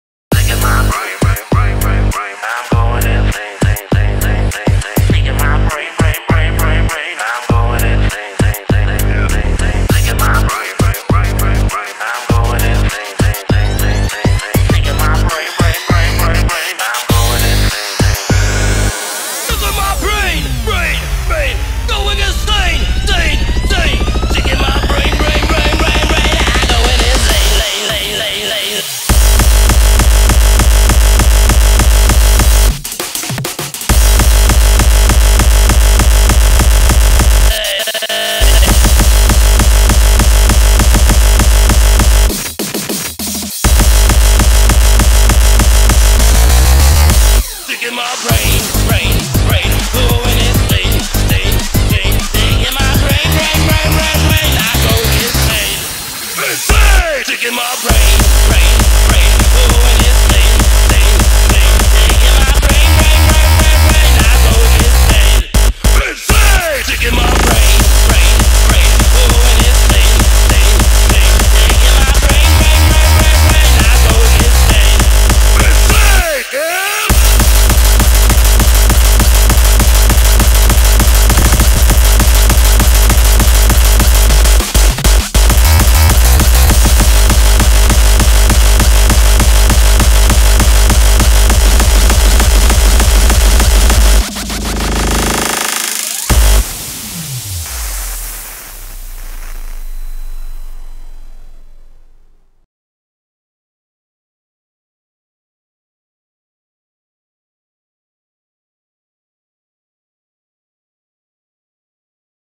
Su Dievu'16 (Aftershot)
Compilation of scenes from annual Christmas Rave in Utena - Su Dievu 2016 (V).